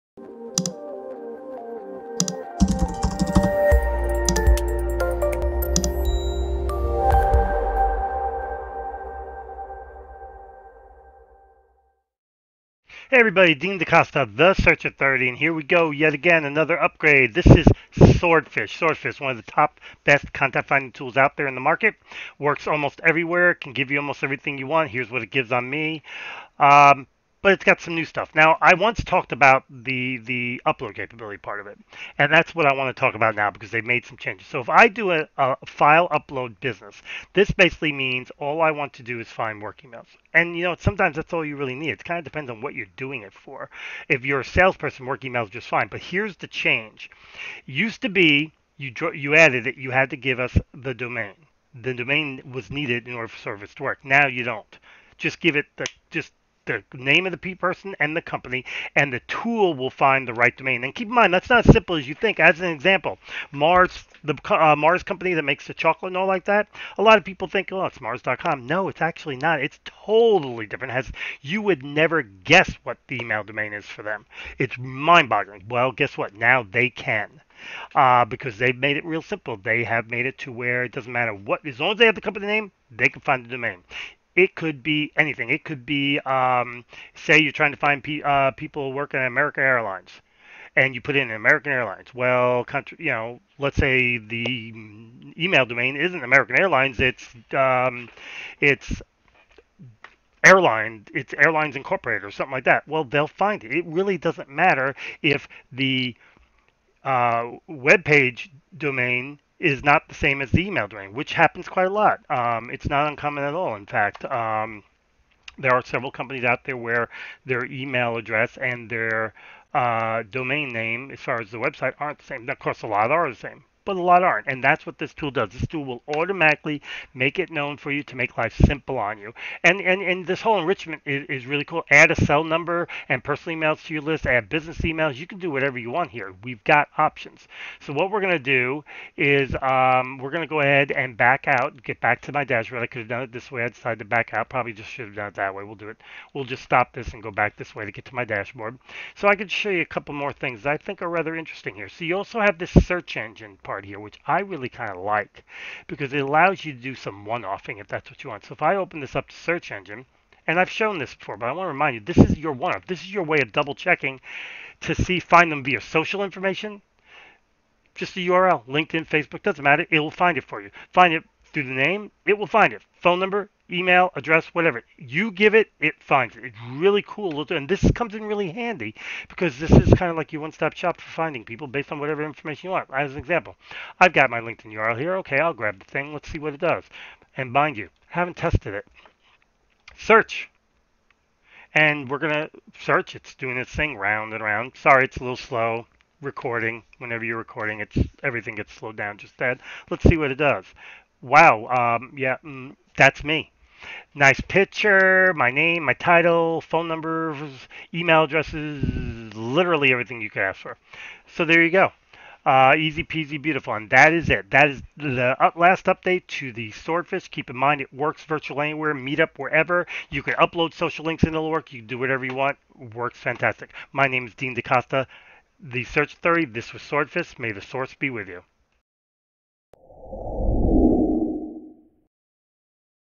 Swordfish Domain Finding
Another upgrade from Swordfish! They added some new stuff.
Before, you needed to provide the domain. Now you don't!
They need the name and the company, and the tool will do the rest.